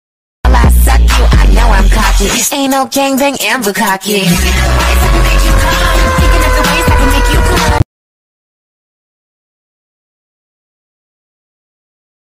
Quick! What's your type?||TREND|| FT. Wanderer||💙🫐 BLOOD WARN!!
🌻Welcome🌻
★Yayyy first video about him. Don't worry I will post more about him and another character!!

🌿Apps Used🌿
•Capcut
•Ibispaint
•Gacha plus

💫Character used💫
•Wanderer
•Me irl